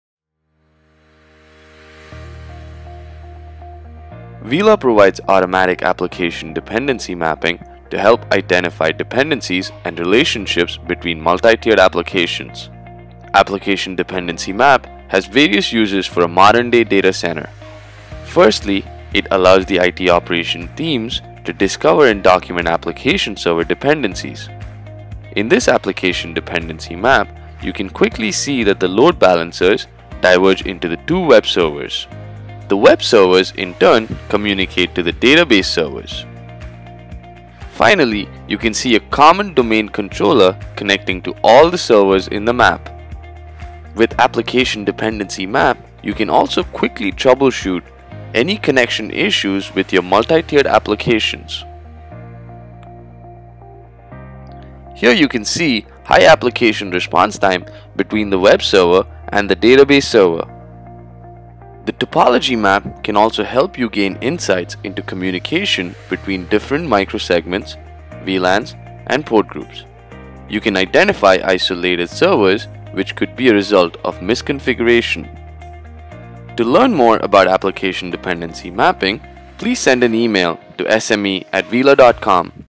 Application Dependency Mapping with Uila
Quick video on how you can Discover & Document Application & Server Dependencies with Uila.